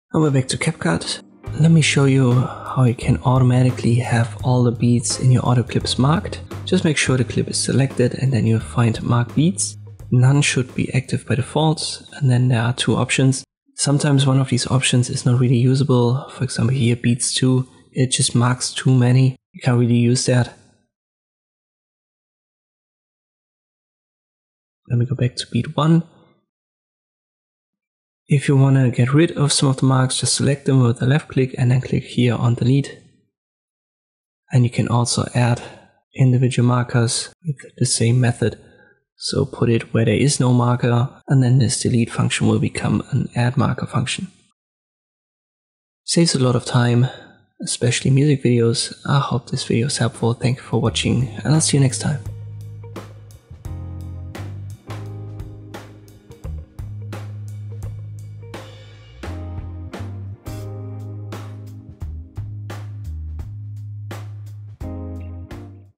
How to Automatically Mark All Beats in a Song in CapCut
In this tutorial I'll show you how to automatically mark all beats in a song in CapCut.